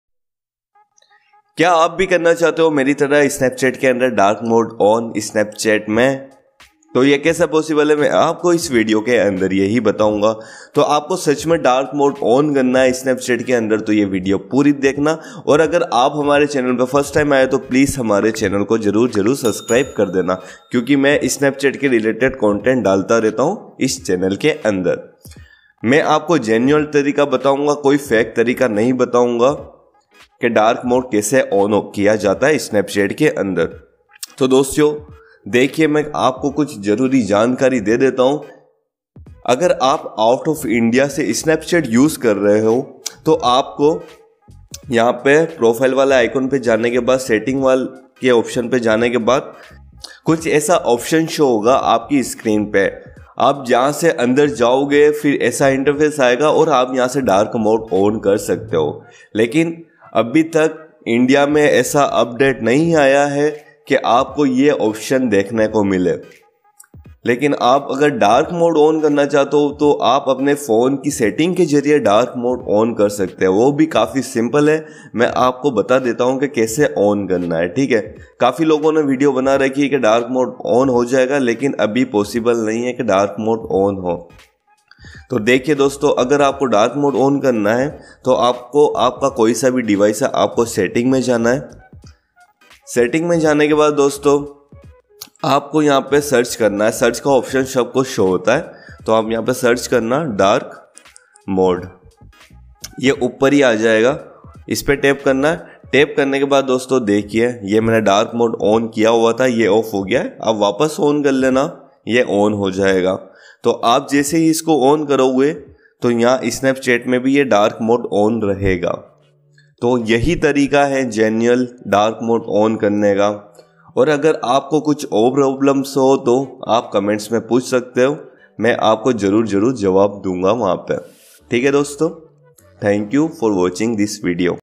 How to enable dark mode on snapchat android | Snapchat me dark mode kaise kare
Snapchat Dark Mode Kaise Kare |  Snapchat Dark Mode | Snapchat Dark Mode Android

Hello dosto ham apko is video me bata rahe hai ki ap apne android phone me dark mode kaise on kare vo bhi sirf 2 minutes ke andar

How to turn on dark mode on Snapchat ANDROID